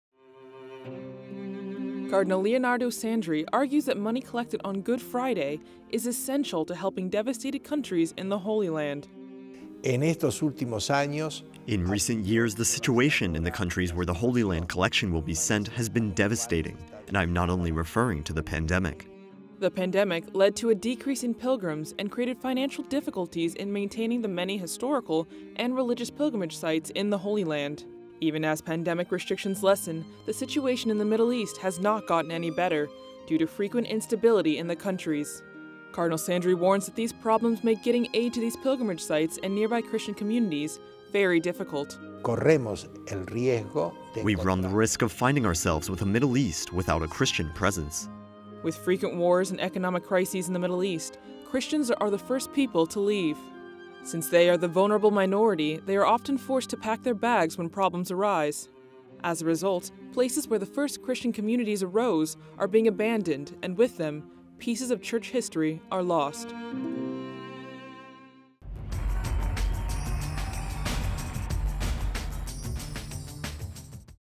CardinalSandri says situation in Holy Land countries is “devastating”
Cardinal LeonardoSandri, Prefect of the Congregation for the Oriental Churches, argues that the money collected on Good Friday is essential to help devastated countries in the HolyLand.

The pandemic has led to a decrease in pilgrimage and financial difficulties in maintaining the religious and historical sites in the Holy East, making it difficult to provide aid. With frequent instability and wars, Christians are leaving the Middle East, putting their presence and their cultural and historical heritage at risk.

Cardinal Sandri warns that the Middle East is at risk of losing its Christian presence.